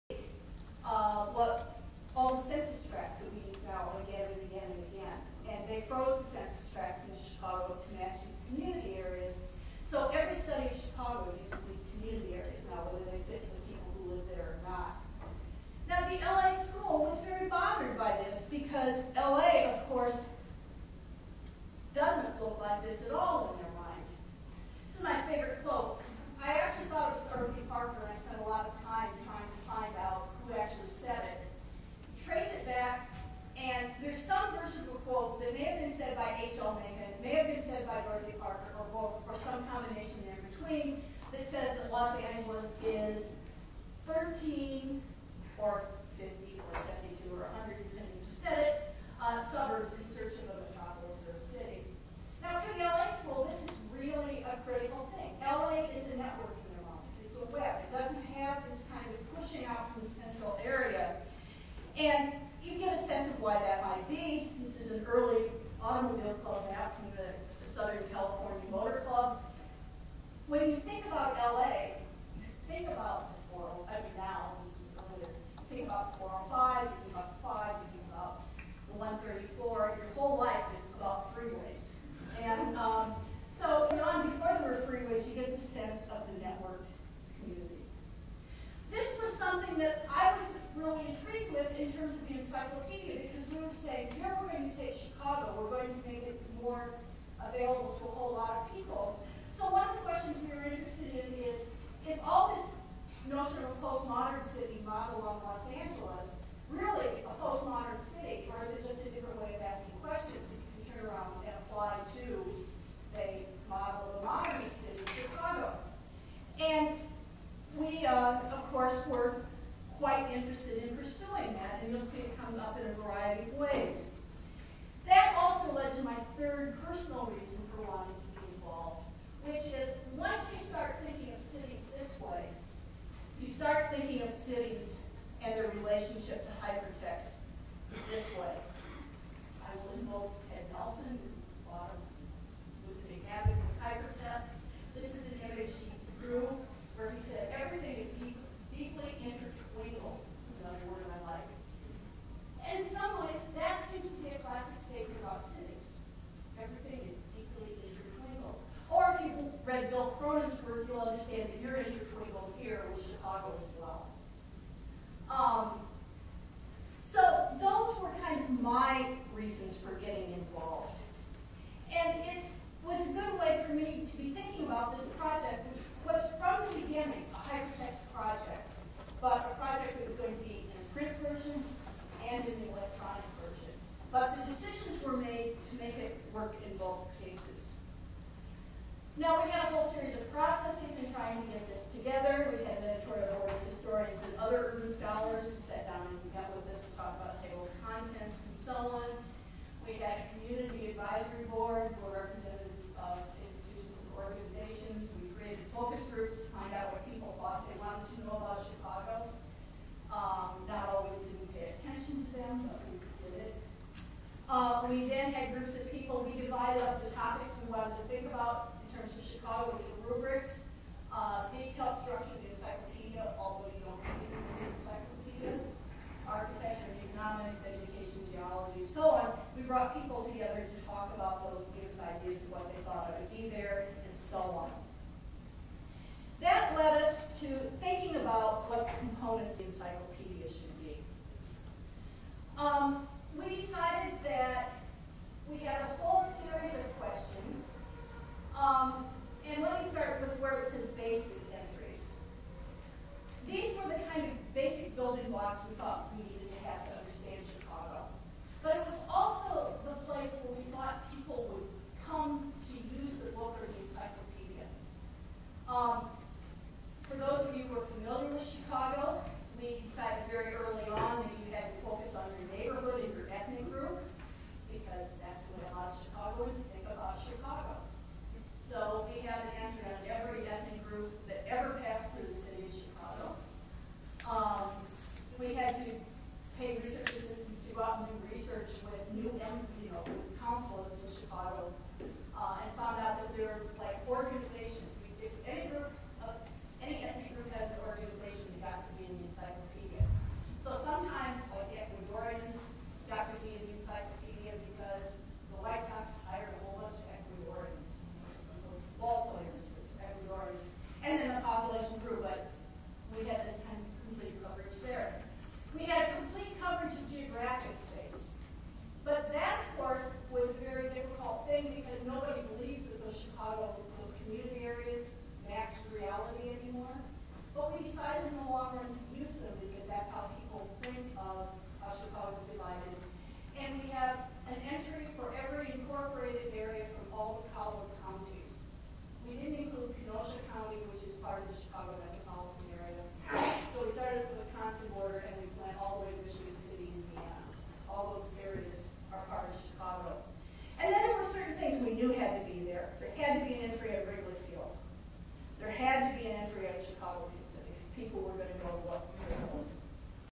Janice L. Reiff Lecture, Segment 2
Segment 2: Networks, "intertwingled" communities, and the structure of the encyclopedia.

Janice L. Reiff of theUniversity of California, Los Angeles presents "Urban Web: Cities as Hypertexts" at the History in the Digital Age Symposium held by the University of Nebraska-Lincoln, September 22, 2006.